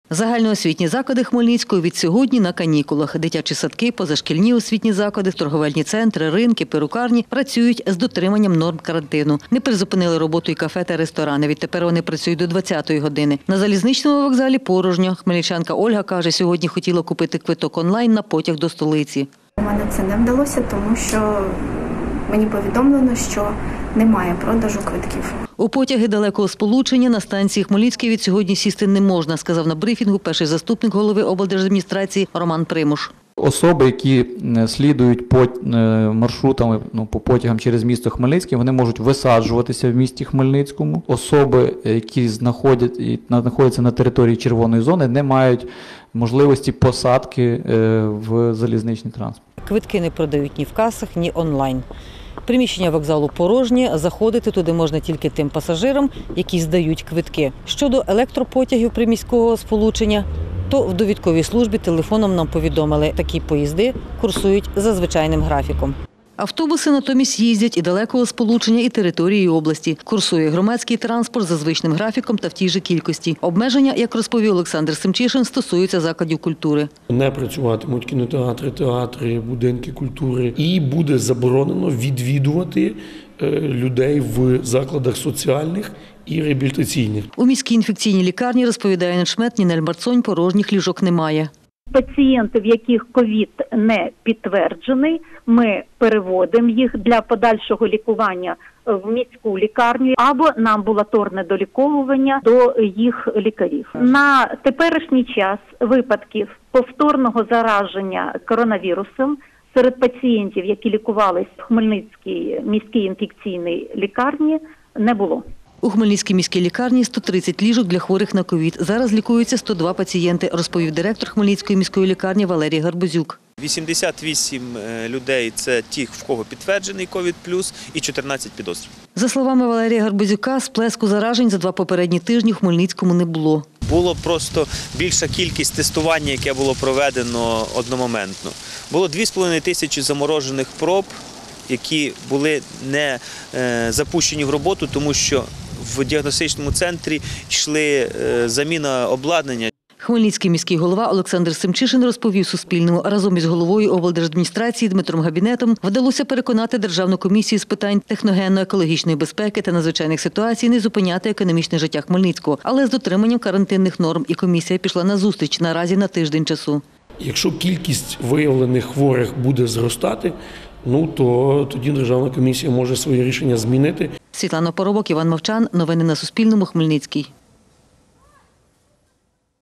Хмельницький у «червоній» зоні: що змінилося
Хмельницький уперше віднесли до червоної зони. Чи зміниться на найближчі два тижні життя містян, та як саме, дізнавалася кореспонденти Суспільного.

Загальноосвітні заклади відсьогодні – на канікулах, дитячі садки, позашкільні освітні заклади, спортивні школи, магазини, торговельні центри, ринки, перукарні — працюють з дотриманням норм карантину. Не призупинили роботу й кафе та ресторани. Щоправда, відтепер вони працюють до 20.00, а не до 22.00, як раніше.